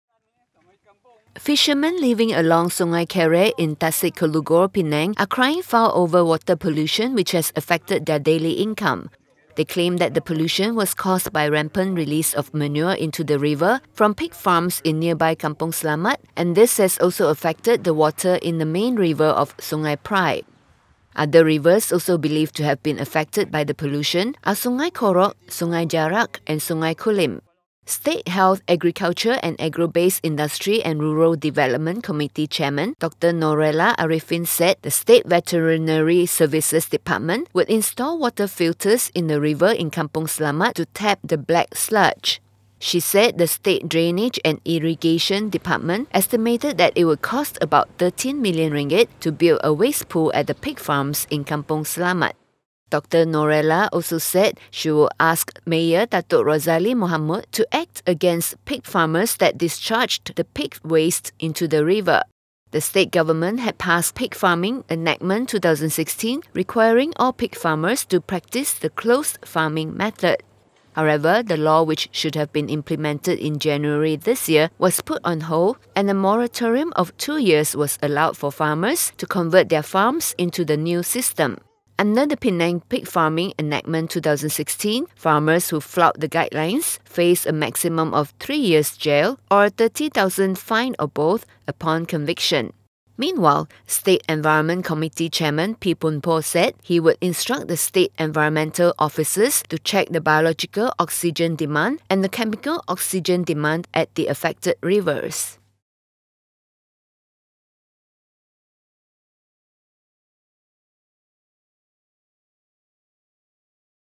Fishermen cry foul over polluted river
Fishermen living along Sungai Kereh in Tasek Gelugor, Penang are crying foul over water pollution which has affected their daily income.

They claimed that the pollution was caused by rampant release of manure into the river from pig farms in nearby Kampung Selamat, and this has also affected the water in the main river of Sungai Prai.